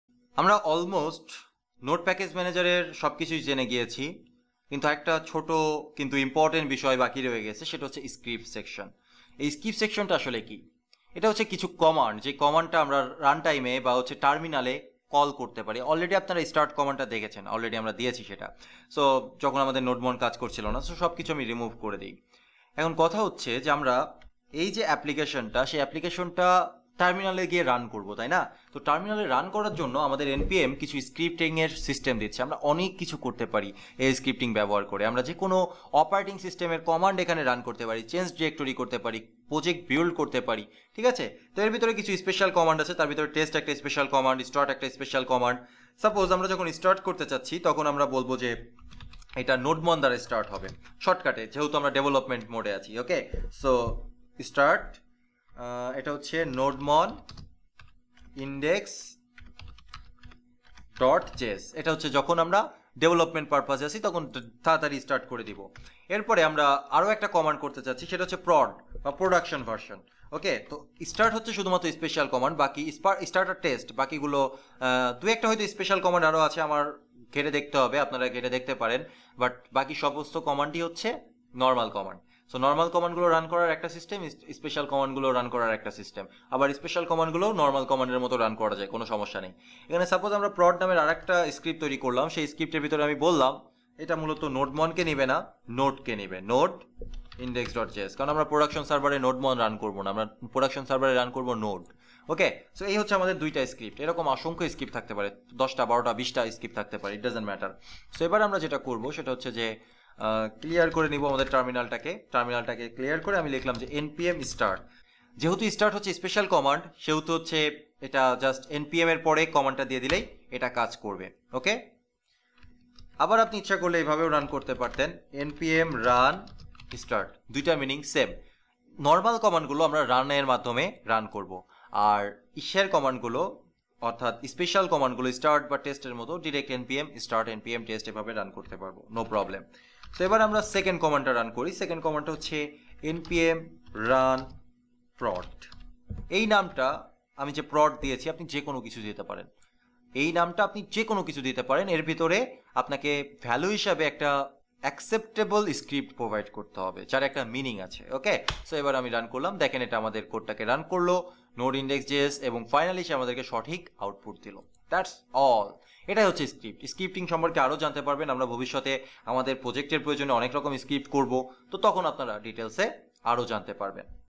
3.9 Write Custom Scripts | NodeJS Bangla Tutorial | Stack Learner
In this lecture, we are going to introduce nodejs. Also, we are talking about our course module and planning. We are going to explore the application we will build together. A multi-author blogging app using nodejs, expressjs, ejs template engines, MongoDB, mongoose, expression sessions and lot more.